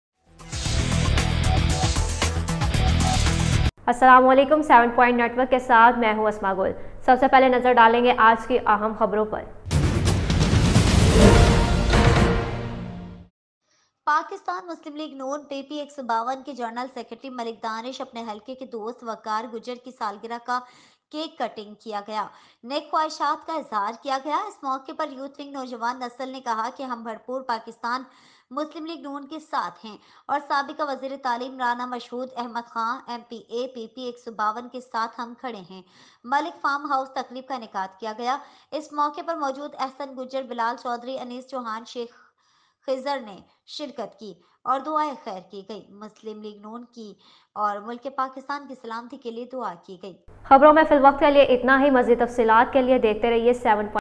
Malik Danish, General Secretary PML-N PP-152, cut Waqar Gujjar's birthday cake.
Pakistan Muslim League-N
 PP-152 General Secretary Malik Danish cut the birthday cake of his constituency friend Waqar Gujjar.  On this occasion, the youth wing of the youth wing said that we are with the Pakistan Muslim League-N and former Education Minister Rana Mashhood Ahmad Khan MPA PP 152.  Ahsan Gujjar Bilal Chaudhry Anis Chauhan Sheikh Khidr present on the occasion attended and prayers were said.
  Prayers were offered for the security of PML-N and Pakistan.